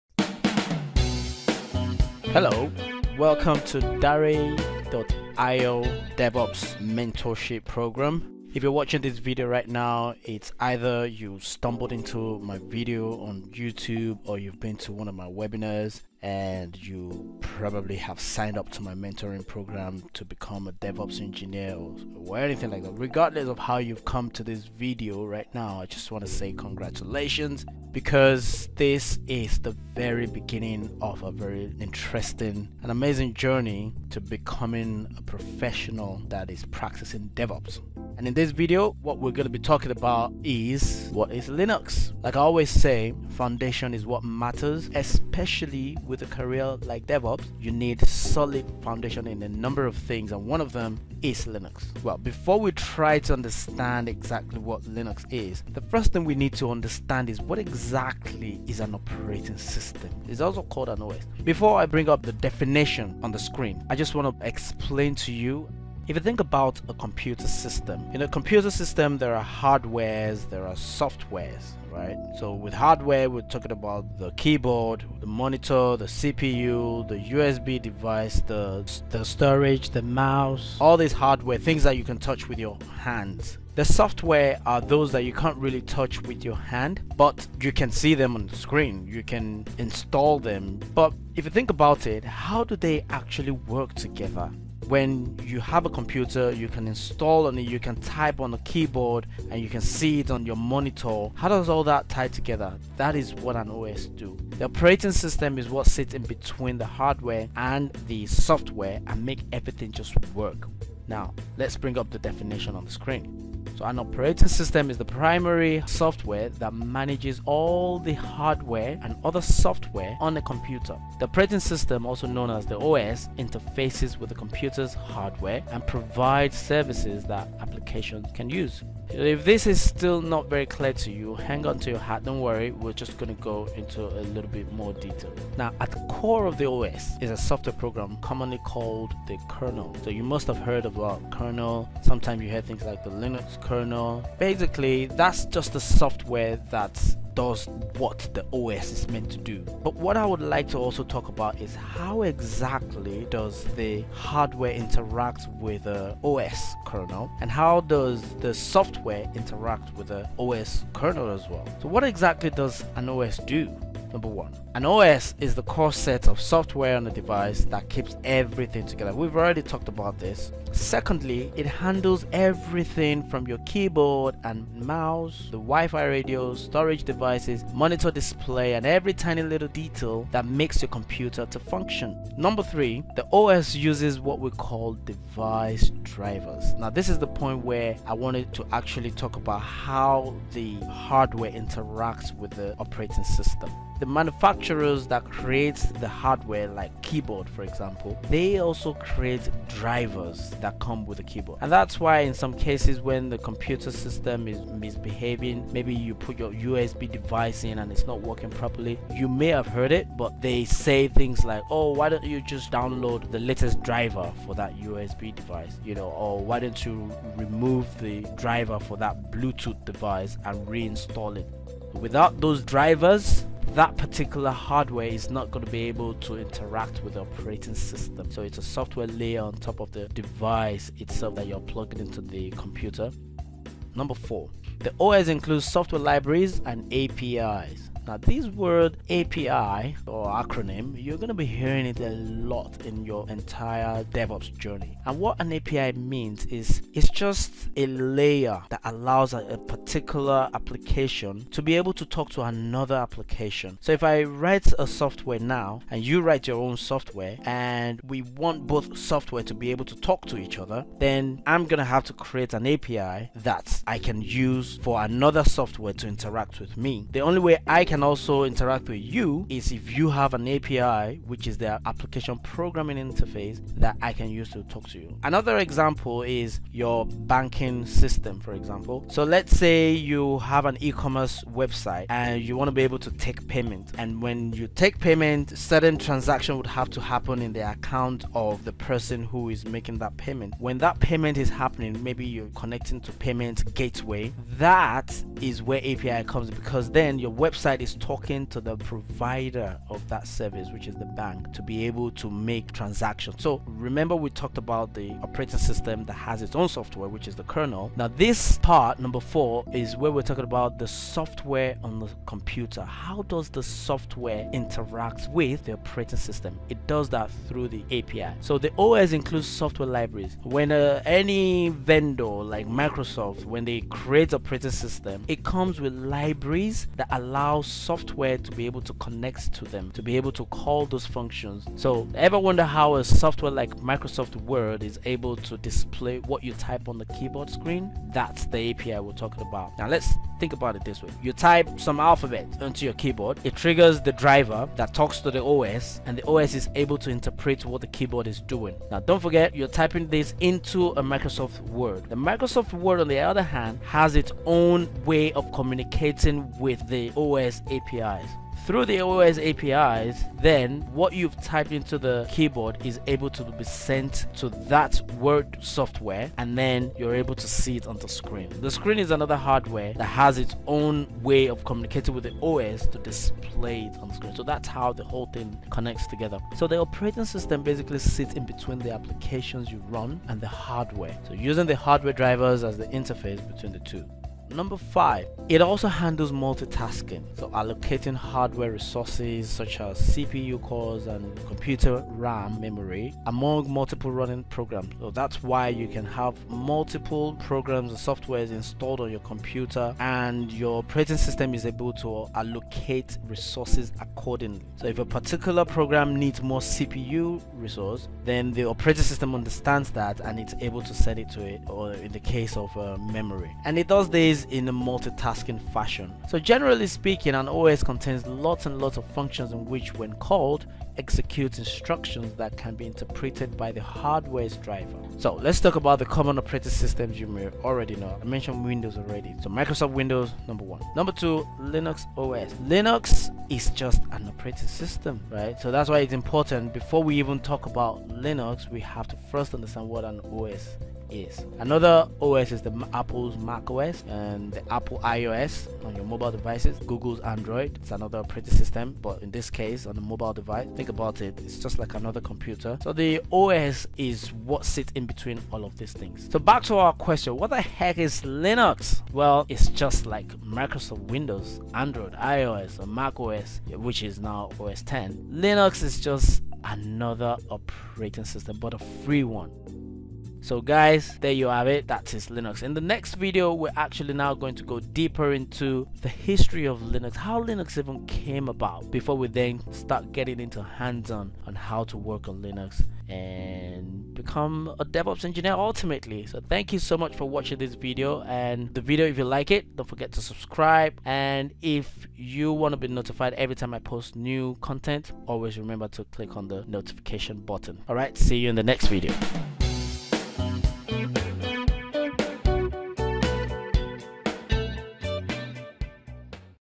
1. What is LINUX
DAREY.IO DevOps Bootcamp & Mentorship Program

If you are considering a career path in DevOps, or Cloud engineering on either AWS, Google, Azure, Digital Ocean, Ali baba, or even in Private Cloud platforms, This video is your very first step to getting technically hands-on as a complete beginner.

Because Linux is the most popular Operating System powering the greatest software programs and websites used by both startups and big companies such as Facebook, Google, Amazon, Netflix, and most banks in the world, It is very important that you understand what it is, before you even think of laying your hands on the terminal.

In less than 8 minutes, you will understand what Linux is, and also get familiar with terminologies such as the kernel, APIs, Libraries, Drivers, etc.

Timestamps:
0:00 Intro
0:55 Understanding OS - Operating System
02:30 Kernel/ Linux (Software)
02:45 What an OS (Operating System) does.
03:15 How the Hardware interact with the OS Operating system and how the software interacts with the OS (Operating System)
03:35 Computer Driver and USB device
04:00 Software Libraries and API's (Application Programming Interface)
05:12 How the Software on a Computer interacts with the OS Operating System
06:34 OS Multi-tasking and allocation of hardware resources
07:06 Common Operating Systems; Microsoft, Linux, macOS, iOS, Google Android,
07:39 LINUX - Free Operating System

In the next video, you will learn more about the history of Linux.